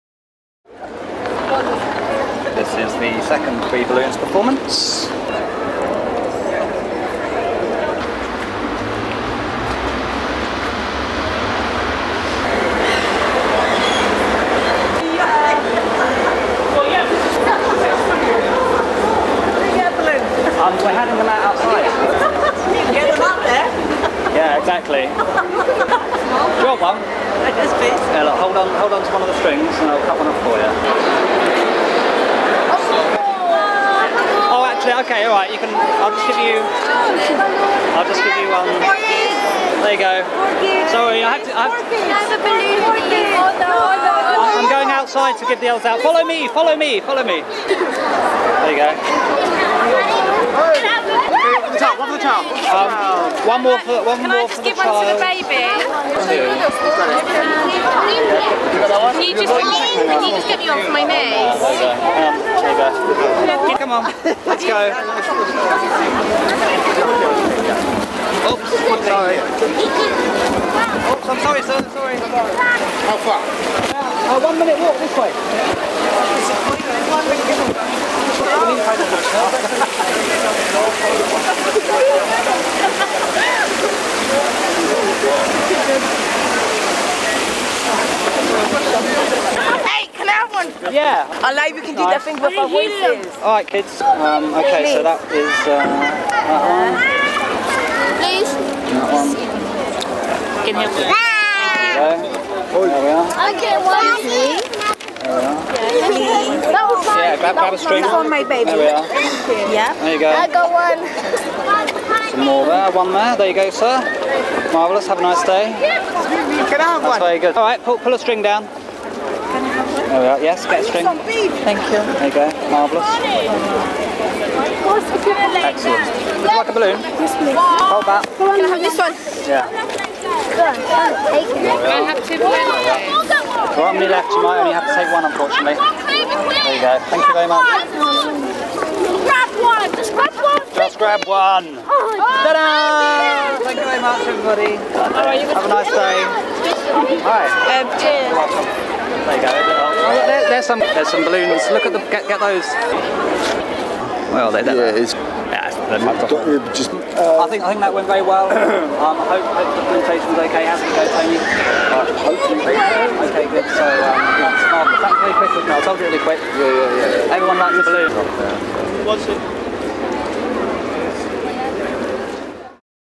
FREE BALLOONS LONDON
30/10/08. To celebrate the opening day of Europe's largest city centre retail project - Westfield London - Richard DeDomenici handed out helium-filled balloons bearing the slogan BORED OF SHOPPING.

Richard's t-shirt read FREE BALLOONS.

FREE BALLOONS is a Bluecoat commission.

Richard DeDomenici is an Artsadmin associate artist.